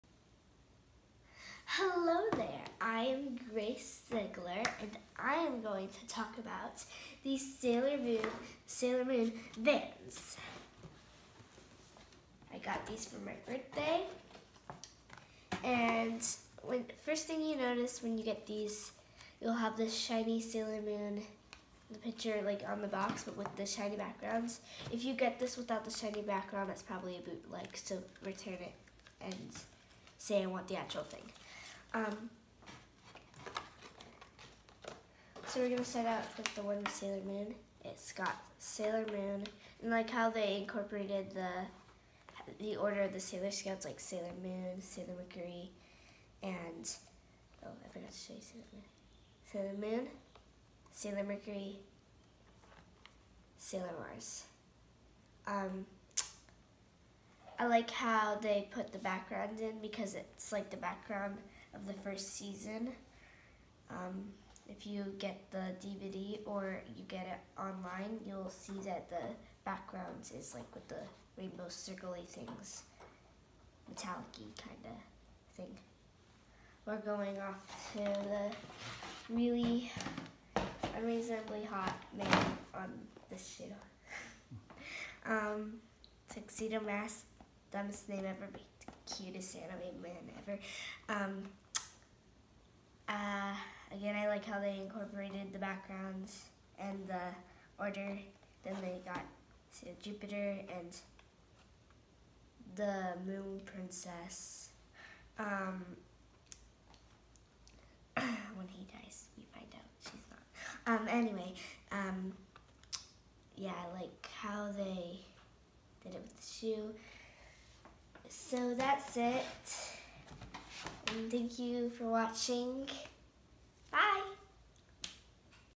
Grace Ziegler talks about her collectible Sailor Moon Vans sneakers
Grace Ziegler got some collectible Sailor Moon Vans sneakers for her 11th birthday and was excited to share them with you.